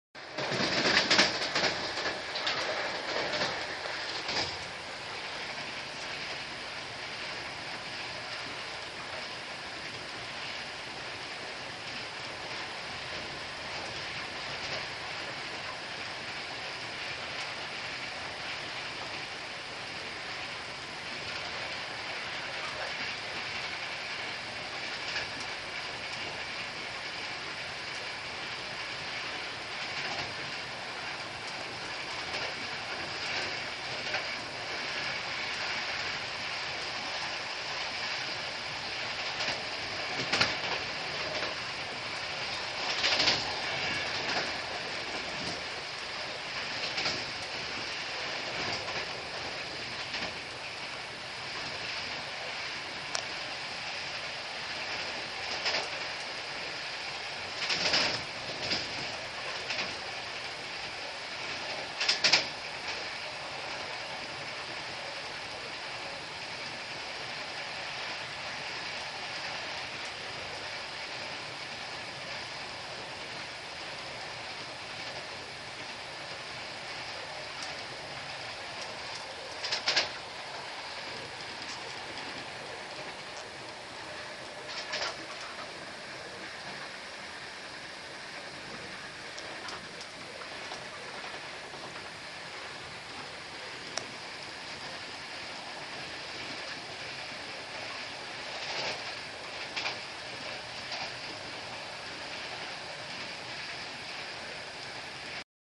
IC Neptun, przejazd przez Mławę 120 km/h
IC Neptun w relacji Warszawa - Gdynia, przedział 1 klasy.